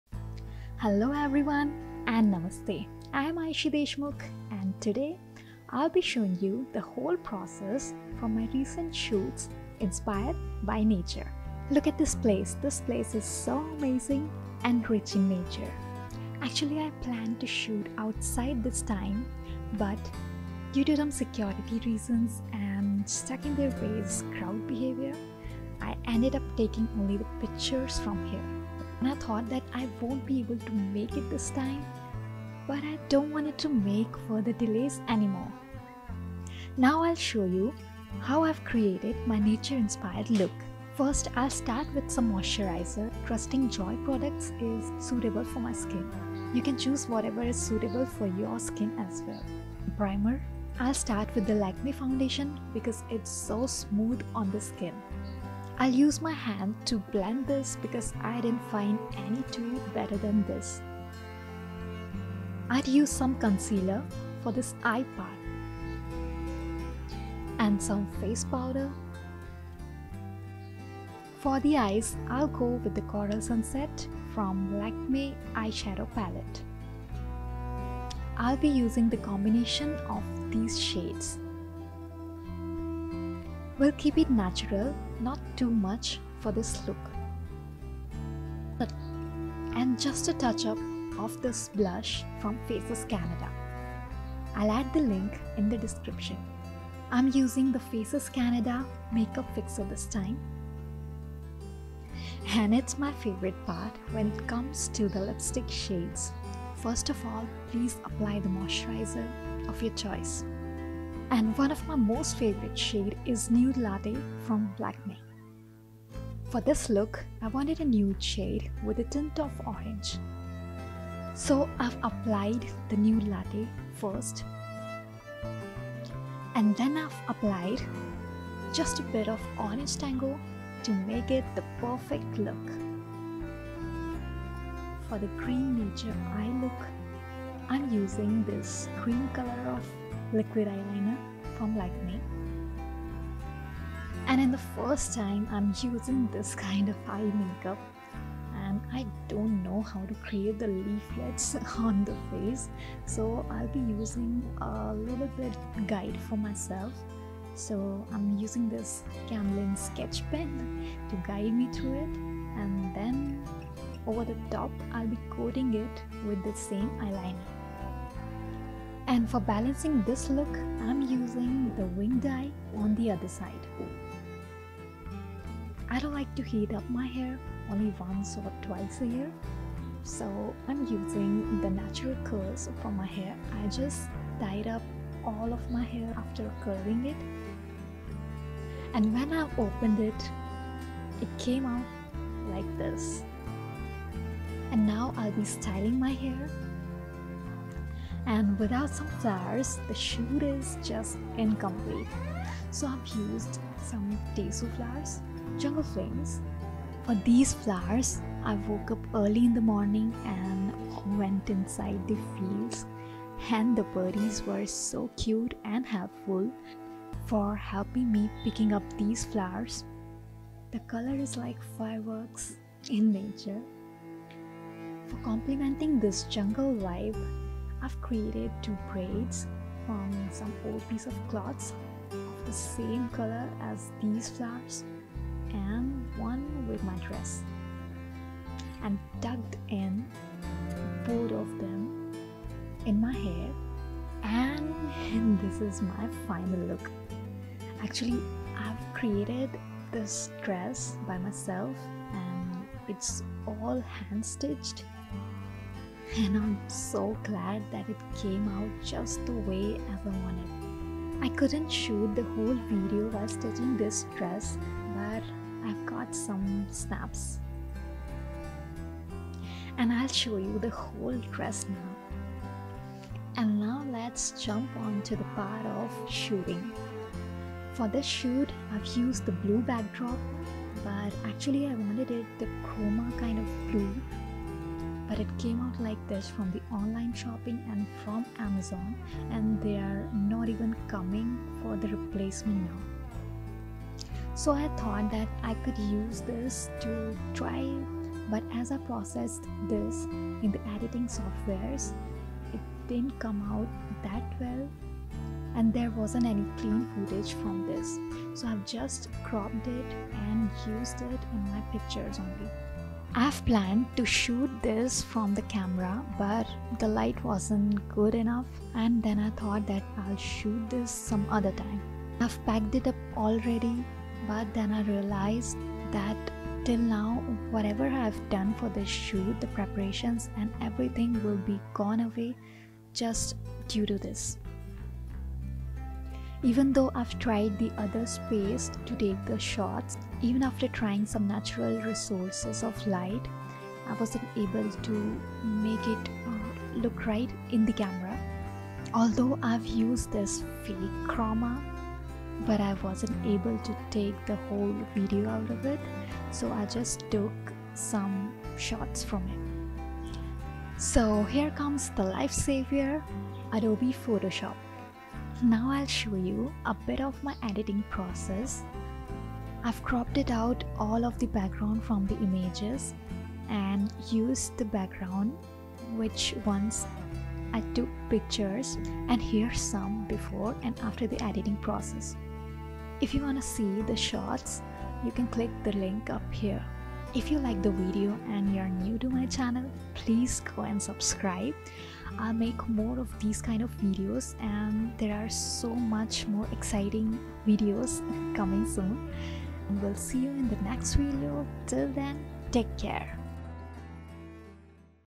From Stitch to shoot: How to create outdoor magic at home?
In this video I'll be showing you how I've created my nature look, designed and hand stitched a dress, took portraits and edited them all by myself.

Equipments : MotoG60 Mobile Camera, Vivo V9 for BTS, Natural light for portrait.
Tripod : Digiteck DTR520BH, Slovic Gorilla Tripod 10 Inch
Editing tools : Adobe Photoshop, Premiere Pro